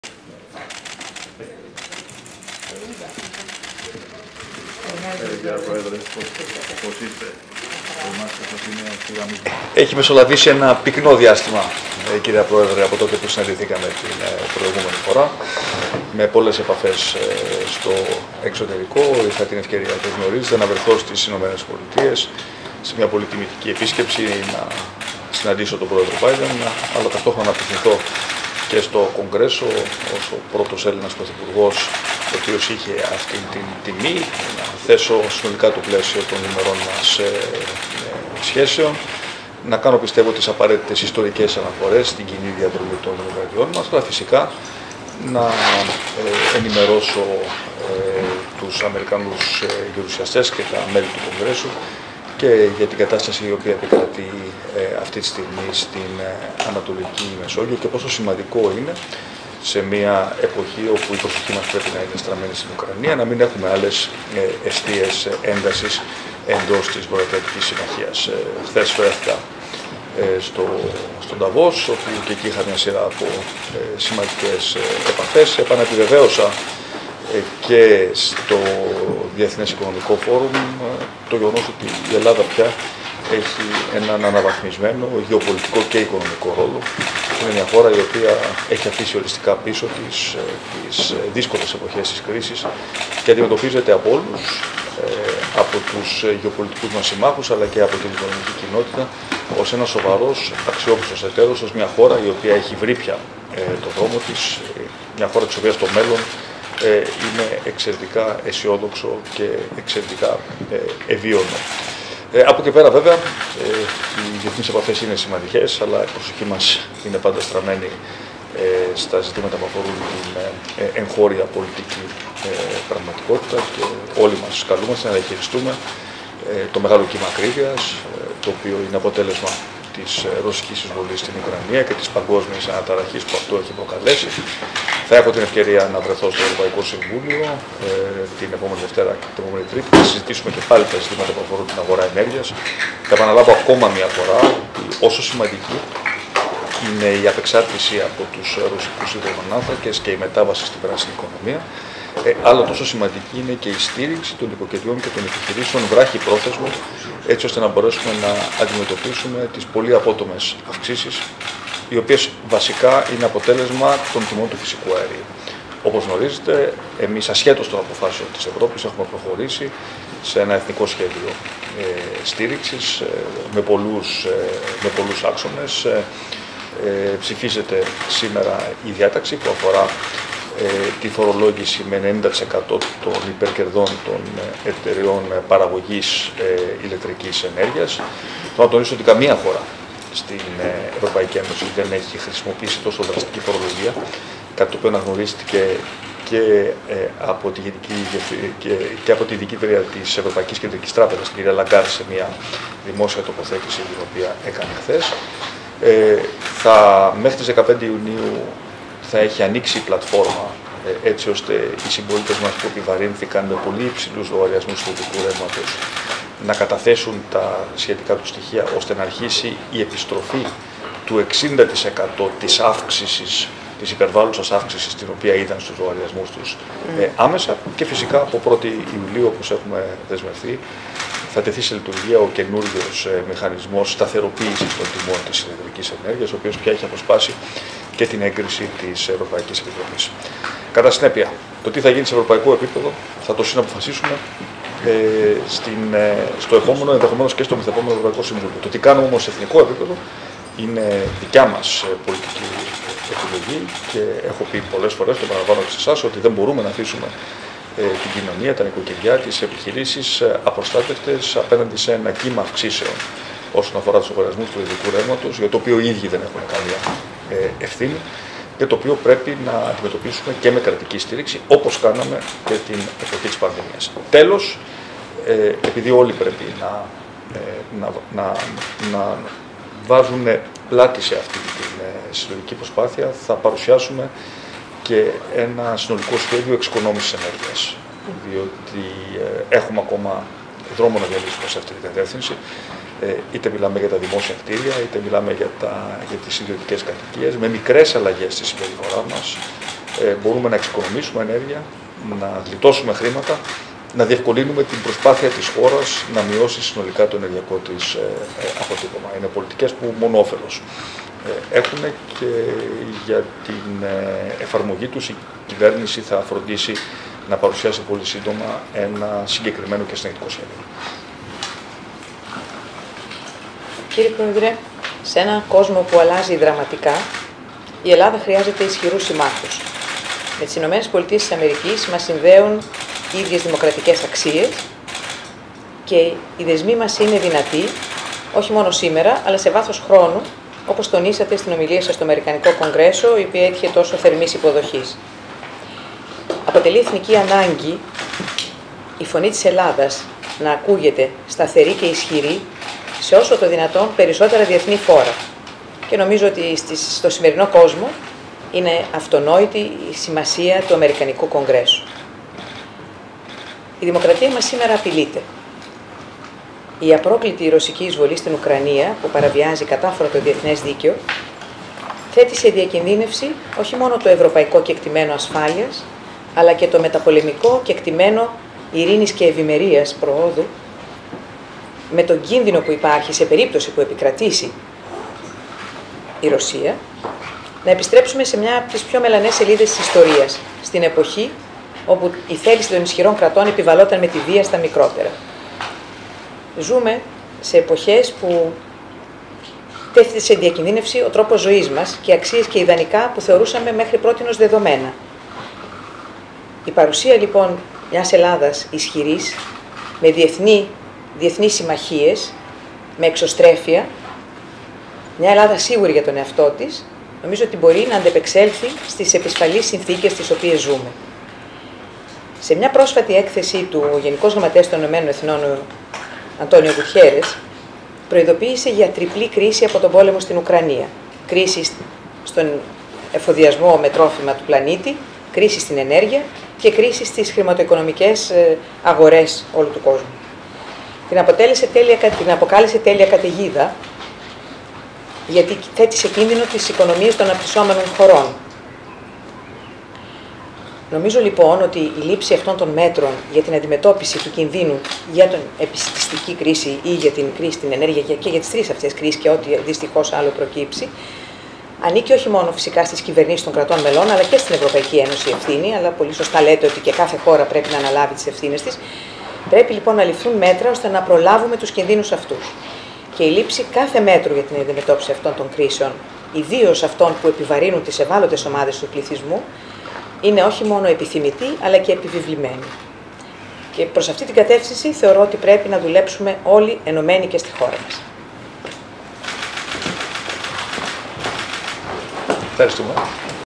Διάλογος του Πρωθυπουργού Κυριάκου Μητσοτάκη με την Πρόεδρο της Δημοκρατίας Κατερίνα Σακελλαροπούλου
Διάλογος του Πρωθυπουργού Κυριάκου Μητσοτάκη με την Πρόεδρο της Δημοκρατίας Κατερίνα Σακελλαροπούλου κατά την έναρξη της συνάντησής τους.